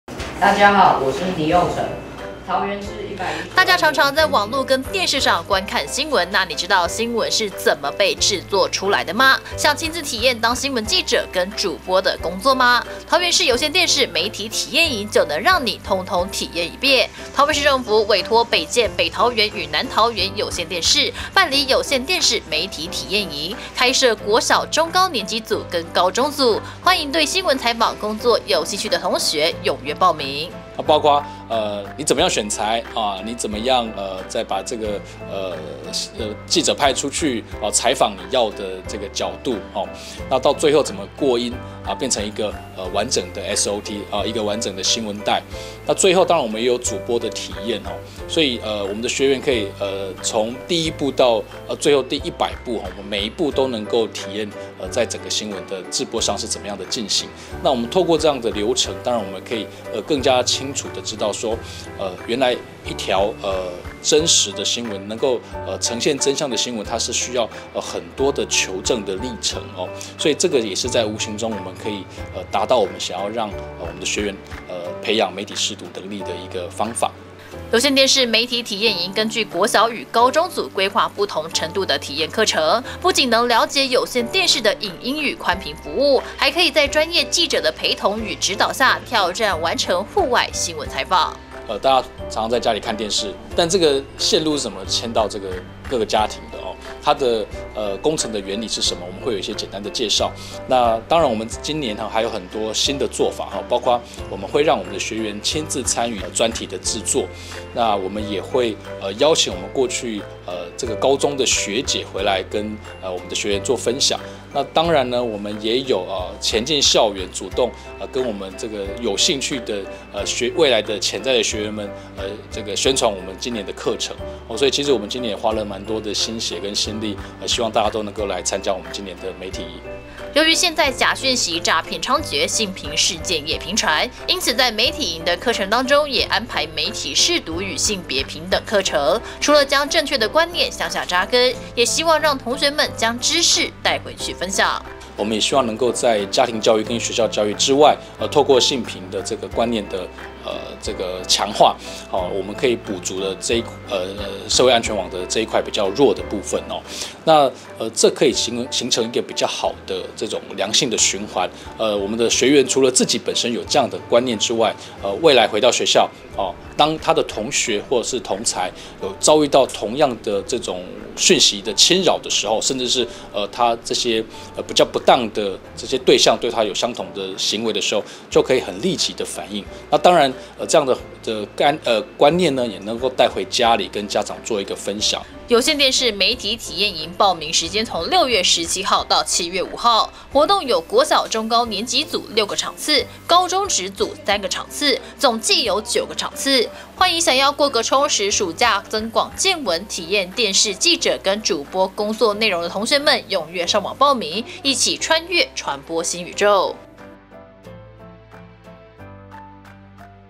桃園有線新聞20240619-穿越傳播新宇宙 有線電視媒體營報名起跑
想要知道專業記者怎麼拍攝、採訪新聞，想要登上主播台播報新聞，認識傳播新科技與培養媒體識讀能力嗎？桃園市政府主辦的有線電視媒體體驗營，從6/17起放報名，開設國小中高年級組與高中組，歡迎對新聞採訪工作有興趣的同學踴躍參加、一起穿越傳播新宇宙。